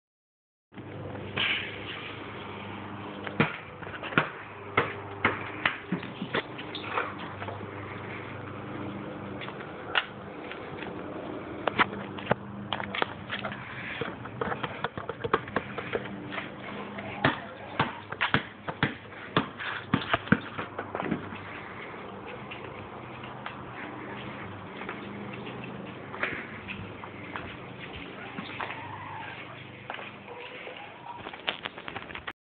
Dog and basketball
He was going nuts over it till I started recording... he saw me bouncing it and he would try to bounce it with his paw